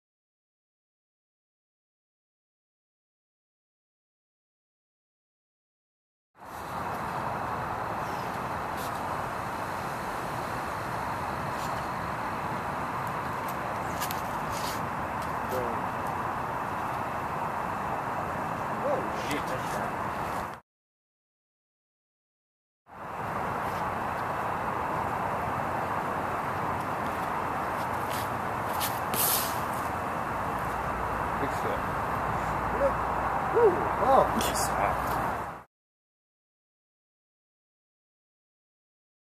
Parking Hole 4 (Lefty & Right Edition)
Tim S. and Chris B. park hole 4 during round 1 of league play.

09/29/2011 - Central Park Disc Golf Course in Oak Brook, IL